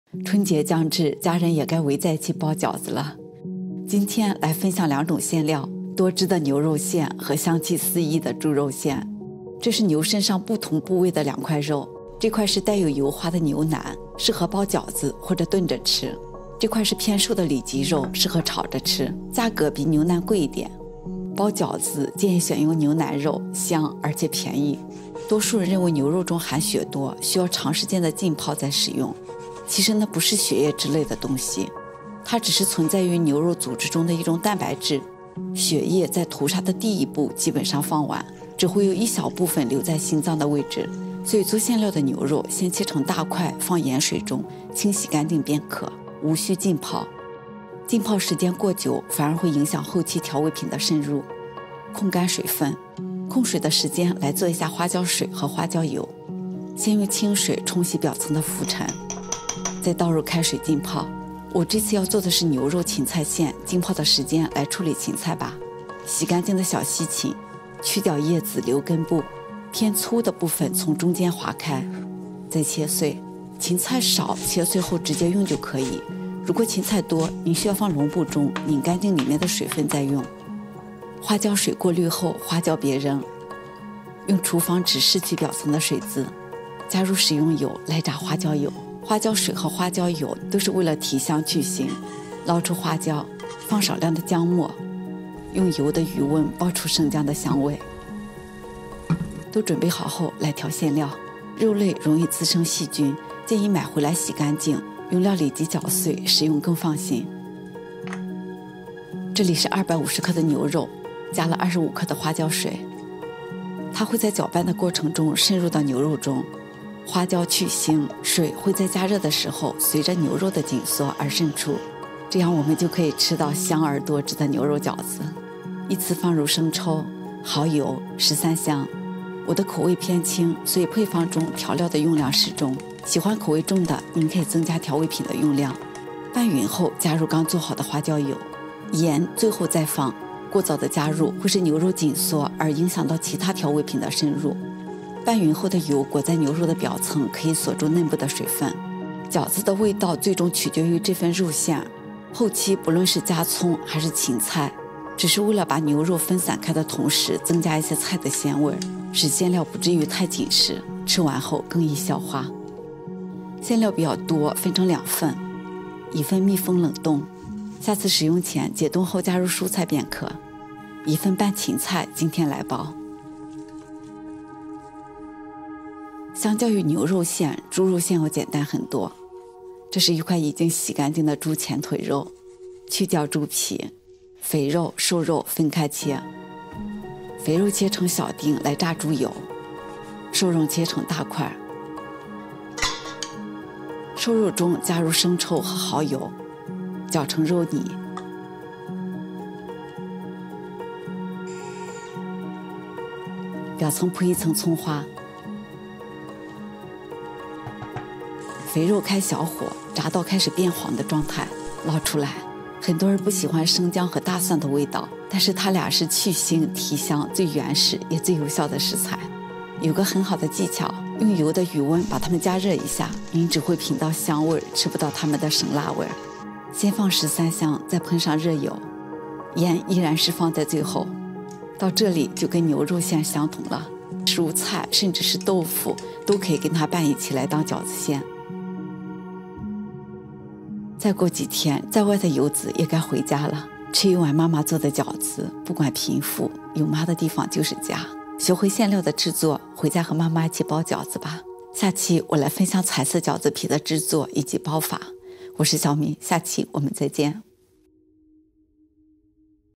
学会这两种馅儿，过年回家和妈妈一起包饺子【小敏的Fun】
牛肉馅：
牛腩肉：250g     花椒水：25g （花椒2g+40g热水）
蚝油：10g           生抽：10g             五香粉：1g
盐：2g                 食用油：35g          姜末：适量
小葱：50g           小芹菜：200g
猪肉馅：
猪前腿肉：250g（去皮，肥肉、瘦肉分开）
蚝油：8g           生抽：8g             五香粉：1g
盐：2g              姜末：适量           蒜末：适量
小葱：40g
小贴士：油热后关火，姜末、蒜末用油的余温爆香